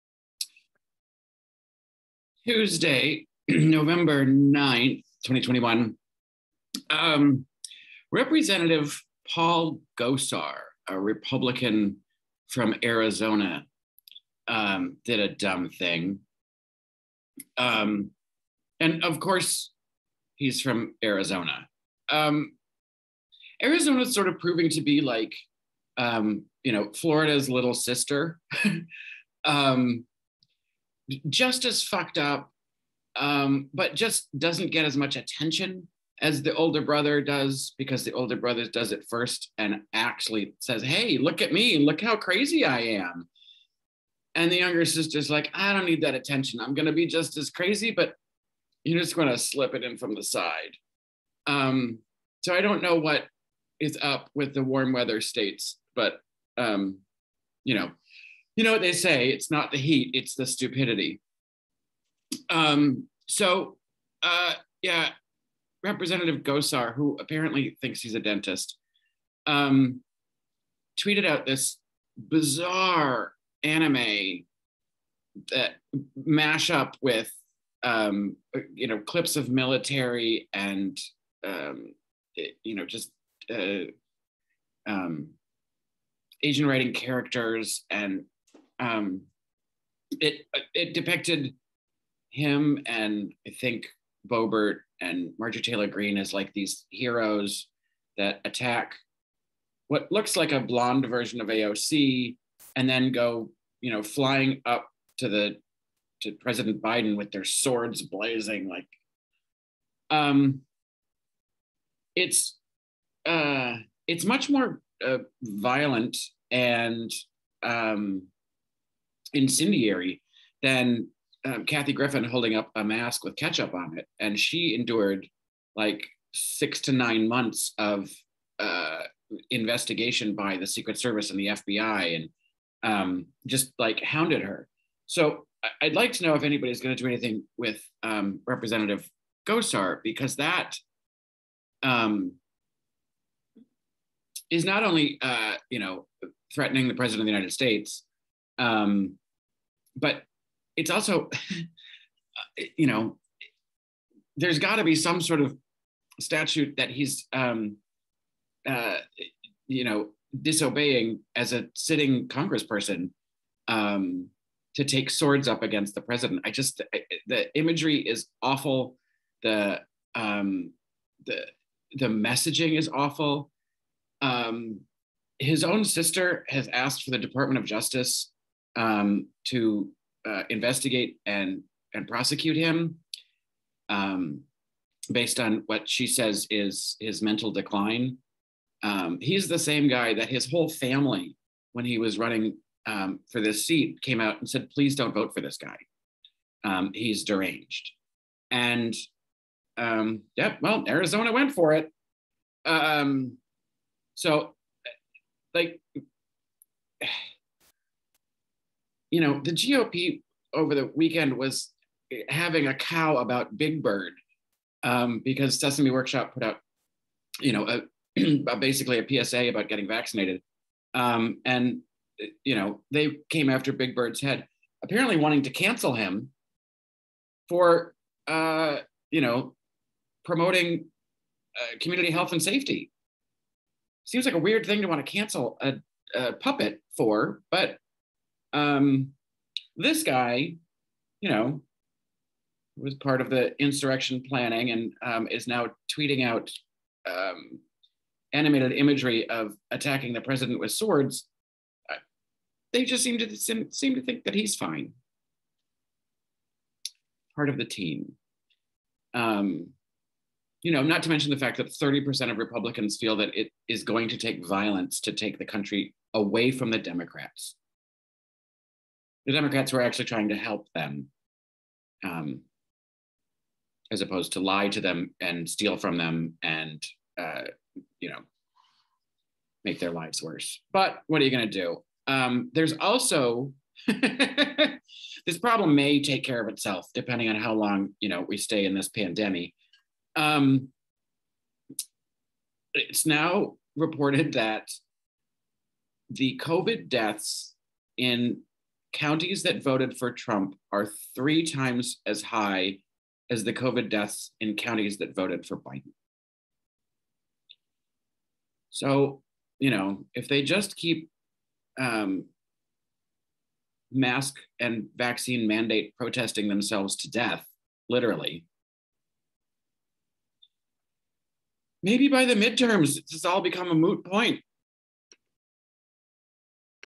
Gosar's Giant Blunder
Rep Paul Gosar tweets an animated video attacking AOC and the PReseident with knives, which kinda seems like a crime. Also, the GOP is dying.
This video is less than a View Hot Topics segment